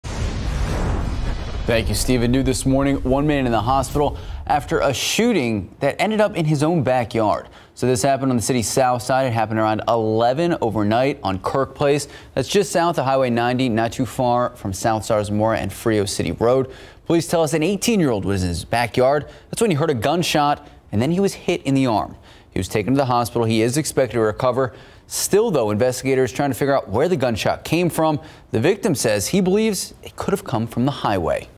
18-year-old in backyard wounded in shooting, police say
An 18-year-old standing in his backyard was wounded in a shooting late Tuesday night, according to San Antonio police.